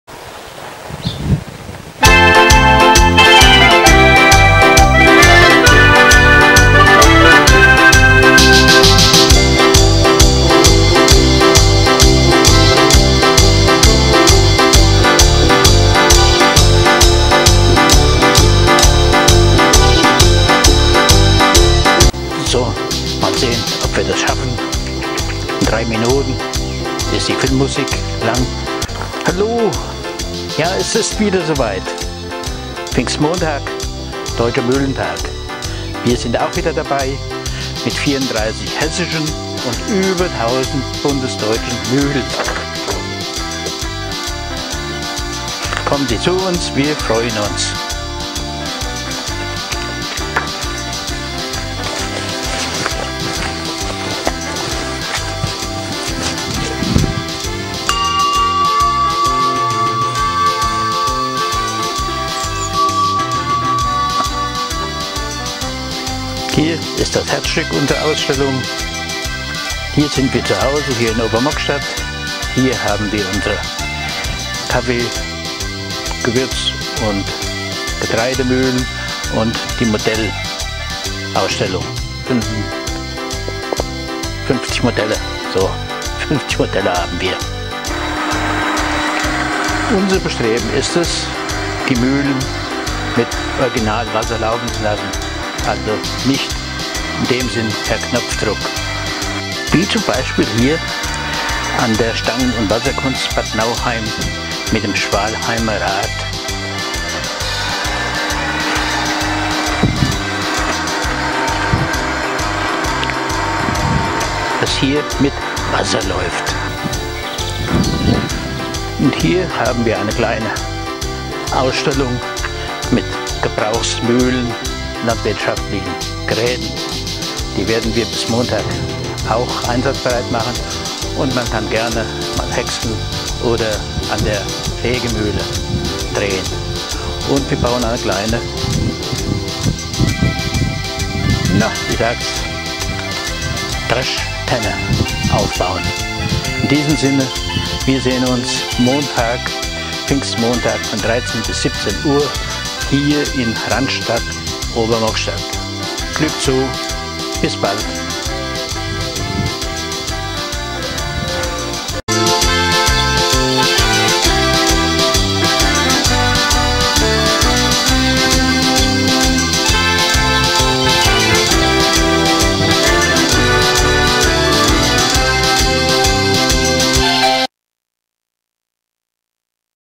Einladung Mühlenausstellung Ob.- Mockstadt am Deutschen Mühlentag 6.6.2022 , (Version Hochdeutsch)
Jedes Jahr gibt es den Mühlentag. Den hat die Deusche Mühlengesellschaft 1994 ins Leben gerufen.. Viele Mühlen deutschlandweit machen da mit und man kann sie an diesem Tag besuchen, Bundesweit sind es weit über tausend, in Hessen sind es 34.. Die Ober- Mockstädter Mühlenausstellung macht da auch mit.. Man kann sie am Pfingstmontag, den 6. Juni 2022 zwischen 13 und 17 Uhr besuchen. Natürlich auch jederzeit, aber nach Anmeldung. Die freuen sich auf ihren / Euren Besuch, wie die anderen auch. .Man sieht sich Ibrigens, wer gerne Dialekt hört, den Fiolm gibt es auch in Platt